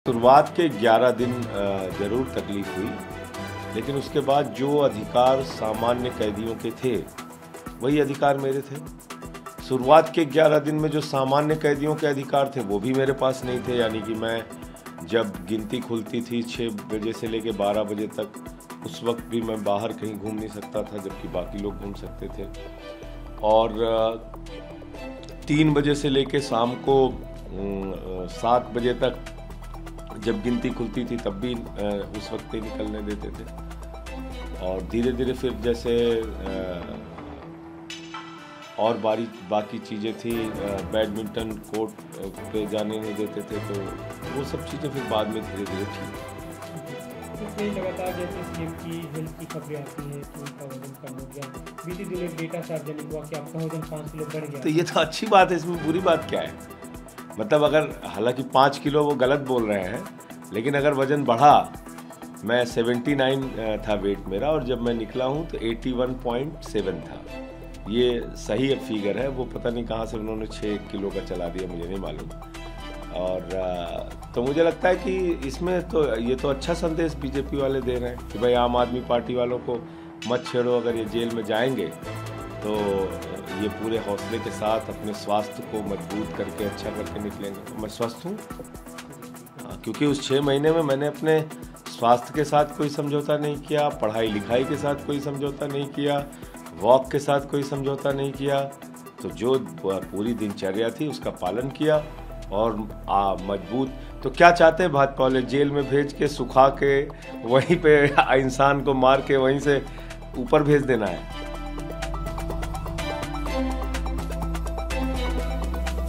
Sanjay Singh shares his Tihar jail experience, says What is wrong if I gain weight…
AAP leader Sanjay Singh narrated about his journey in jail. Over the discussions of his weight gain during his captive days, the AAP leader said that the initial days at the jail were tough and later he gained weight. Watch to know more!